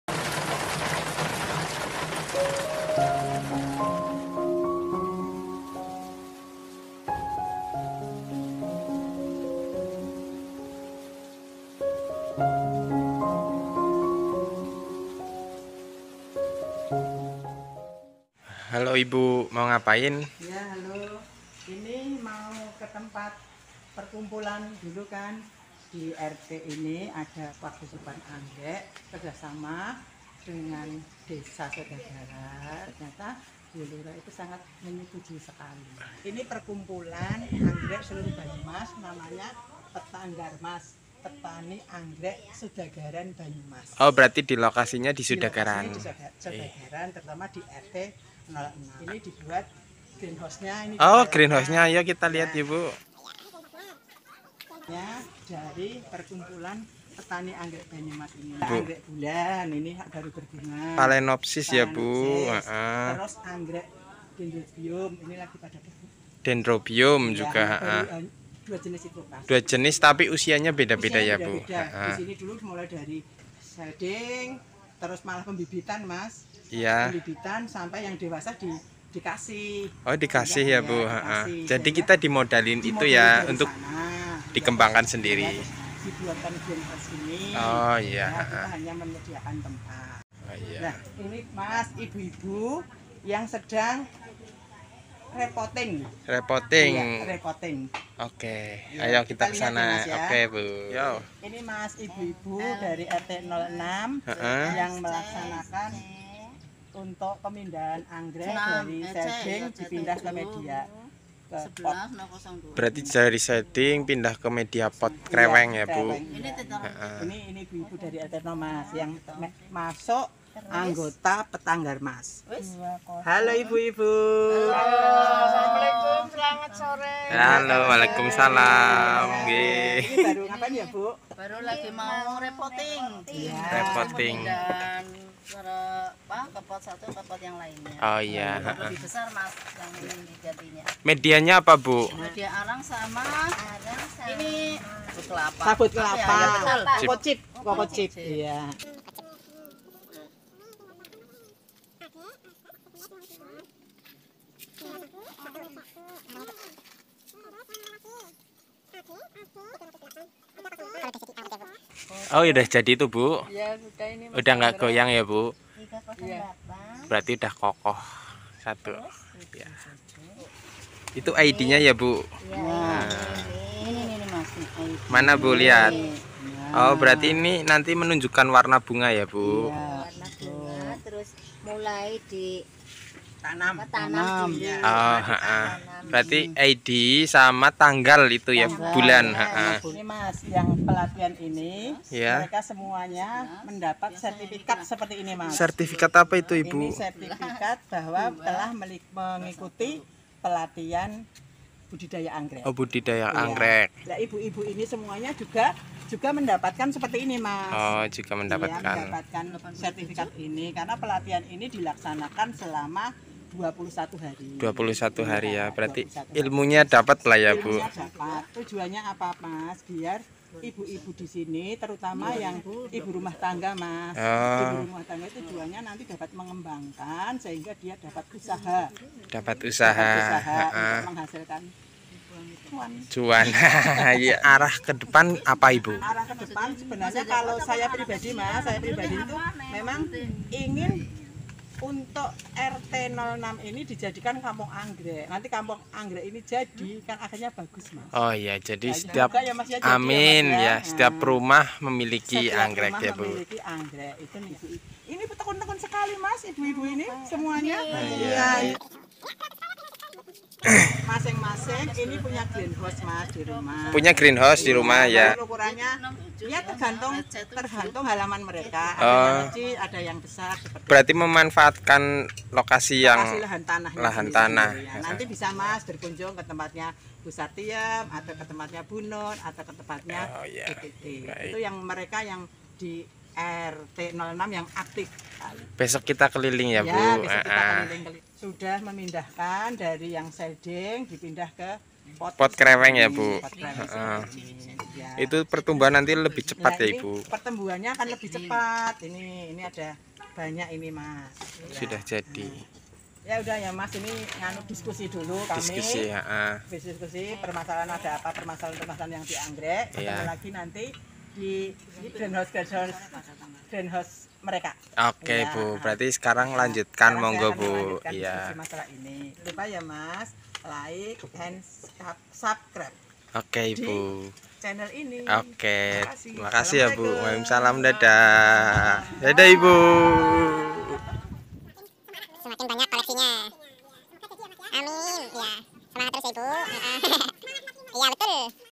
Perawatan Anggrek Bersama Kelompok Pertanian Anggrek Banyumas
Hai Teman-teman...

Video kali ini kita bersama kelompok petani anggrek sudagaran banyumas melakukan perawatan rutin anggrek di green House kelompok.

Sekilas info!!
Kelompok ini dibentuk karena cita2 kami untuk menjadikan desa kami menjadi kampung anggrek, pemerintah desa sangat mendukung cita2 kami sebagai kampung wisata budidaya anggrek.. agar terwujud cita2 kami, dilakukan pelatihan budidaya anggrek oleh Balai Pelatihan Kerja dan Transmigrasi Provinsi Jateng pada tahun 2021. Dari situlah terbentuknya kelompok petani anggrek dan hingga saat ini perawatan anggrek masih rutin dilaksanakan oleh anggota2 dan selalu berkonsultasi dengan Jeng Endang Orchid..

Arah kedepan adalah membagi ilmu kepada semua warga agar paham mengenai budidaya anggrek dan disetiap rumah warga diharapkan memiliki tanaman anggrek

INFO KONTAK:

Shopee : jengendang.orchid

Store: Desa Sudagaran, Kec. Banyumas,  Kab. Banyumas,  Jawa Tengah